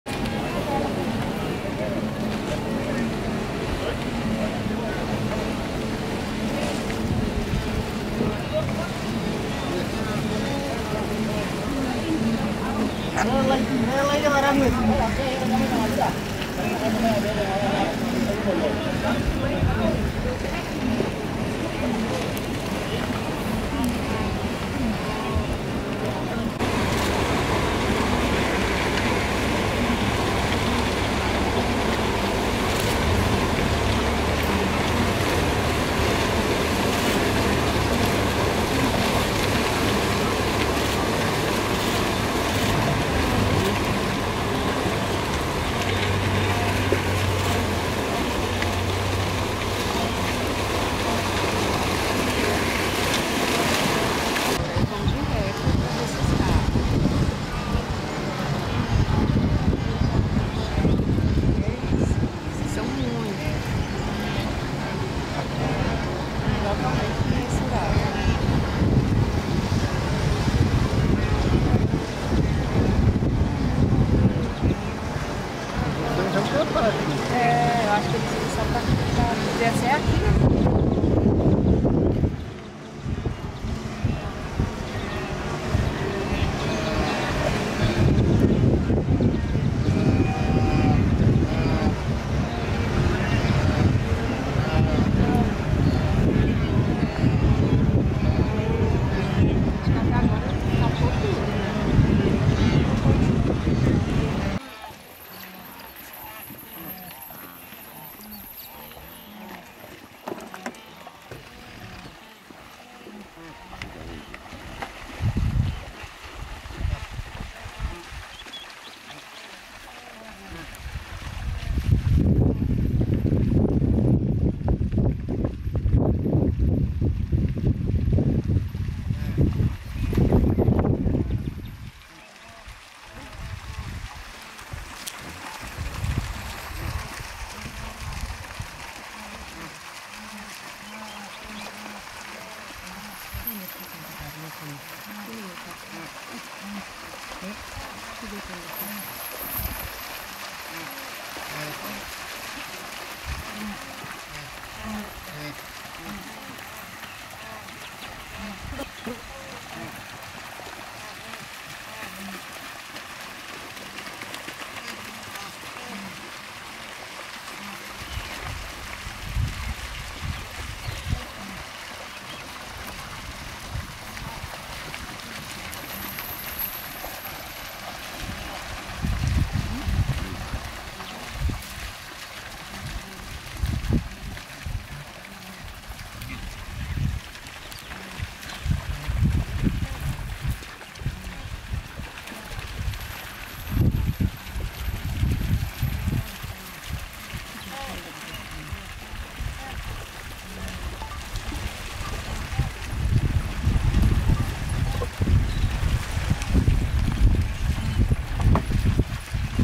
Witness an extraordinary Migration of the Wildebeest across the Mara River: An Epic Natural Wonder!
Experience the incredible power of nature with this amazing footage of the wildebeest migration! Witness the awe-inspiring spectacle of thousands of wildebeest migrating in unison from Maasai Mara National reserve to Serengeti across the Mara River. Follow their journey as they brave the challenges of the treacherous terrain and the perilous predators of the savanna. This is a breathtaking journey that you won't want to miss!